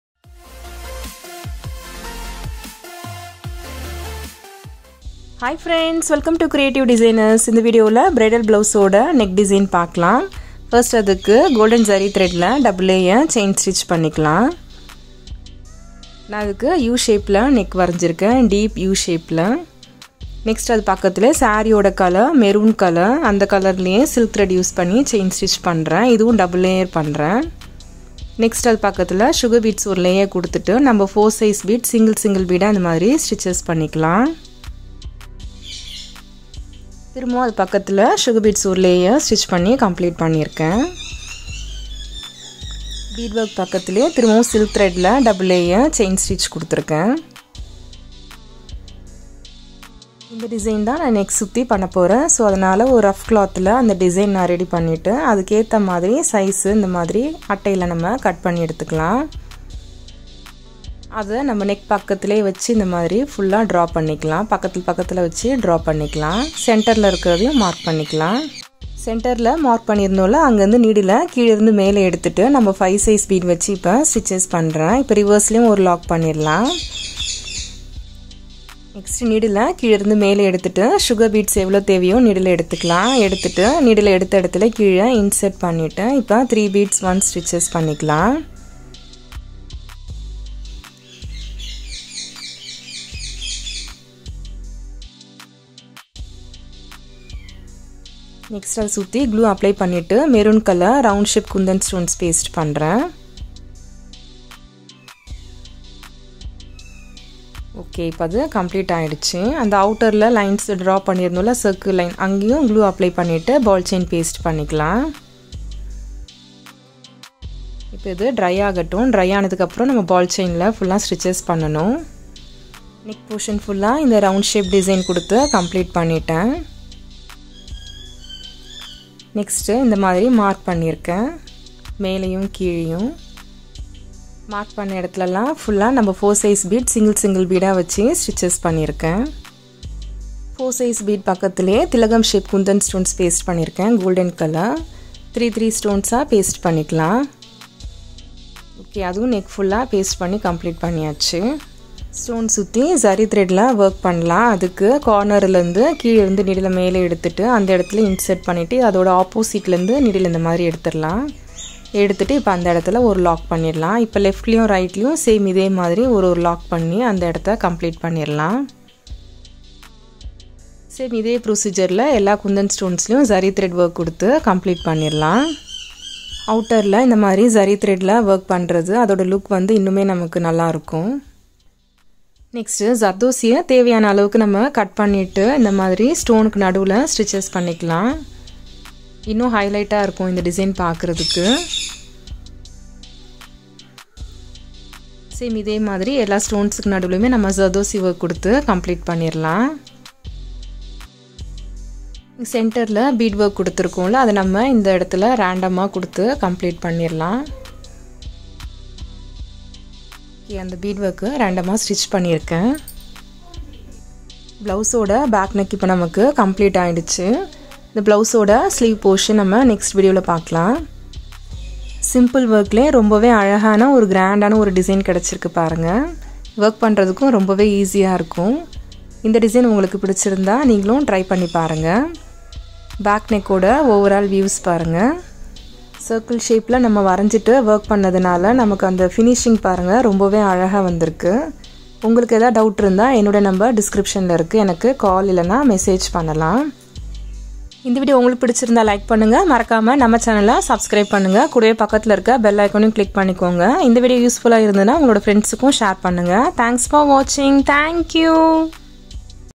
Aari bridal neck design |Easy bridal neck design for beginners|Creative designers
Hi friends, in this video you gonna see my latest aari bridal neck design which is very useful for beginners.

Call or Whatsapp @ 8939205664 to shoot your doubts.